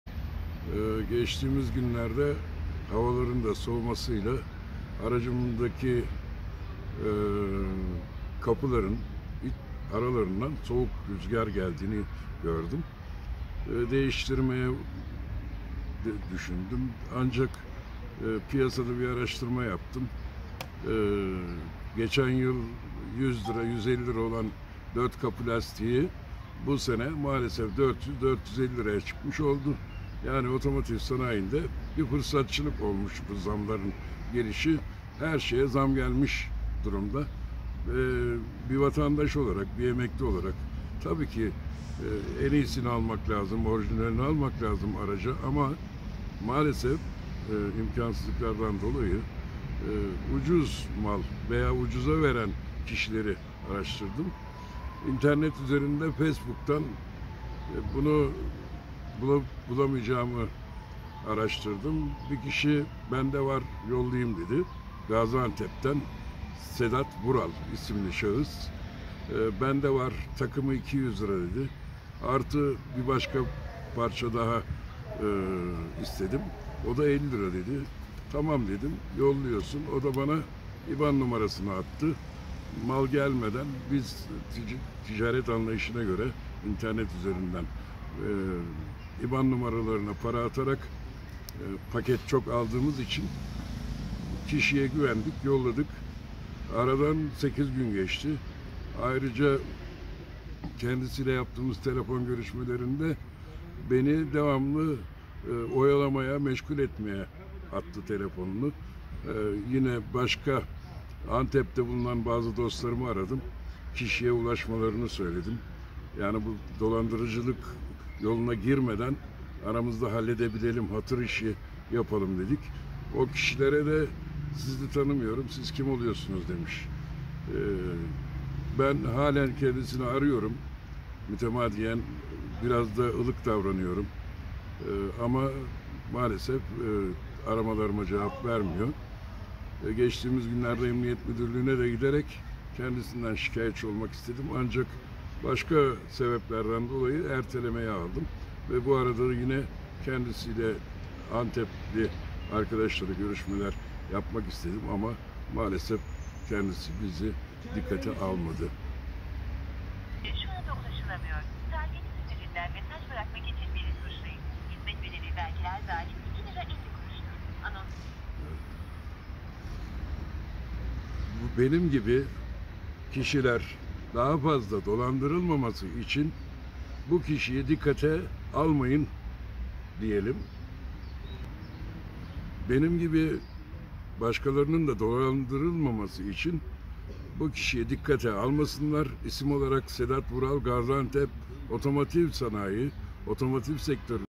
Sosyal medya üzerinden dolandırıdı
Tümer araştırmaları sonucunda, profilinde otomotiv parçası işi yaptığını belirten ve Gaziantep'te olduğunu iddia eden bir vatandaşa ulaştı.

Vatandaştan toplamda 250 liralık otomotiv parçası sipariş veren ve parayı iban numarasına gönderen Tümer'e aradan 8 gün geçmesine rağmen herhangi bir kargo paketi ulaşmadı. Otomotiv parçası sattığını iddia eden şahıs telefonlara da cevap vermeyince Savaş Tümer dolandırıldığını anladı.

İnegöllü vatandaşların, kendisi gibi dolandırılmamaları için açıklama yapan Tümer şu ifadeleri kullandı

PARAYI GÖNDERDİ AMA ÜRÜNLER GELMEDİ
"Geçtiğimiz günlerde havaların da soğumasıyla birlikte aracımdaki kapıların arasından soğuk rüzgar geldiğini gördüm. Değiştirmeyi düşündüm ancak piyasadaki yaptığım araştırmalarda, geçen yıl 100 lira olan kapı lastiklerinin bu yıl 400 lirayı geçtiğini gördüm. Tabi ki en iyisini almak lazım araca ama maalesef emekli bir vatandaş olarak ucuza veren kişileri araştırdım. İnternet üzerinden araştırdım. Facebook platformunda Gaziantep'de bulunan bir kişiye ulaştım. Şahıs bende var takımı 200 lira dedi. Bir başka parça daha istedim 50 lira istedi. İban numarası attı ve parayı gönderdim. Aradan 8 gün geçti fakat ürünlerim gelmedi. Kendisiyle yaptığım telefon görüşmelerinde de beni hep oyaladı. Halen kendisini arıyorum ama maalesef aramalarıma cevap vermiyor. Benim gibi kişilerin daha fazla dolandırlmaması için bu kişiyi dikkate almayın."